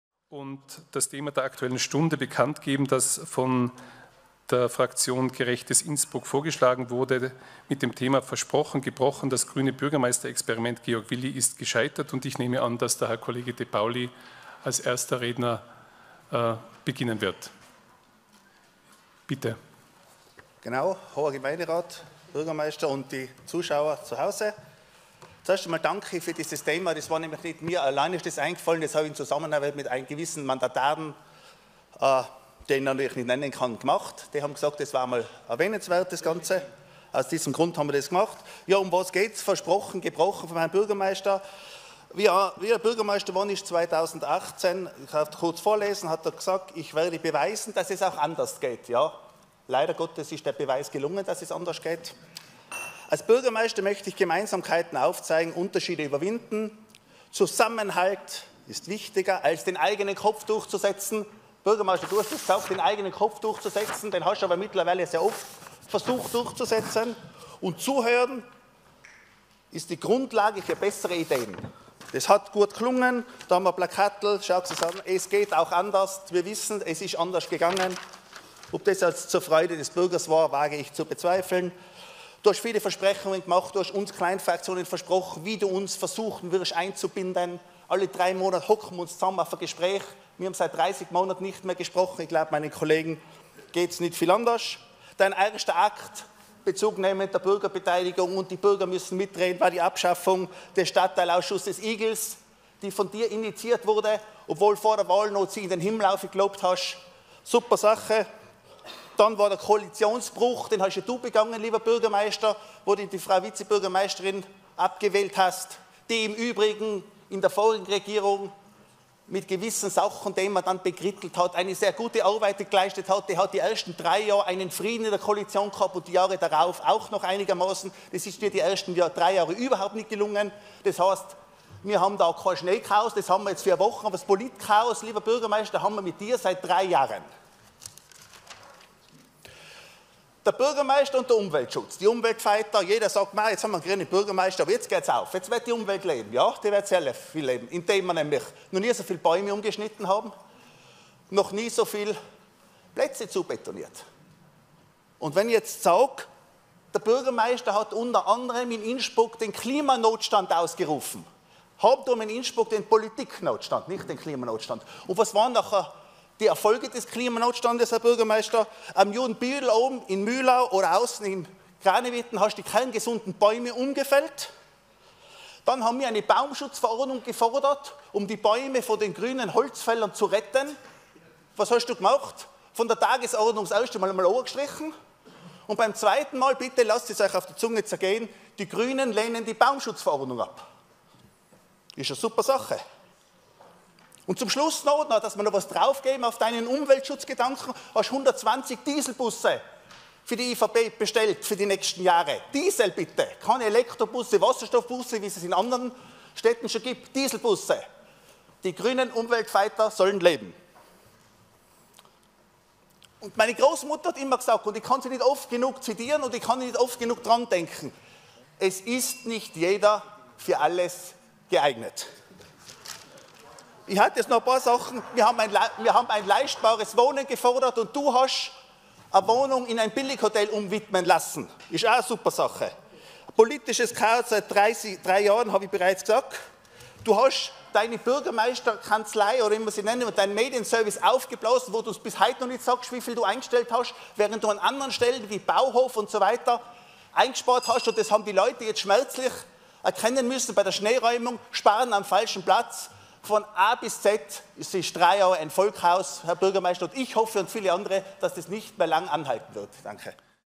Versprochen gebrochen - Das grüne Bürgermeisterexperiment Georg Willi ist gescheitert
Unsere Rede zur Aktuelle Stunde.
Aus der Gemeinderatssitzung vom 21 Jänner .
- KRITISCH - SCHONUNGSLOS,
und leider der WAHRHEIT entsprechend !

Schnitt: Markus Kandler